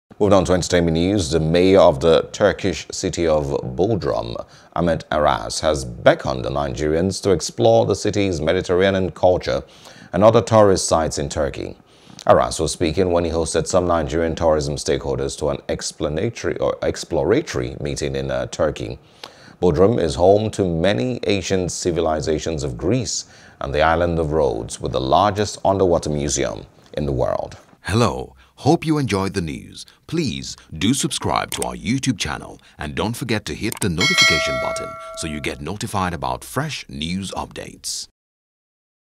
Mayor Of Bodrum Woos Nigerian Tourists To Explore Its Mediterranean Culture | ENTERTAINMENT
The Mayor of the Turkish city of Bodrum, Ahmet Aras, has beckoned on Nigerians to explore the city’s mediterranean culture and other tourists’ sites in Turkey.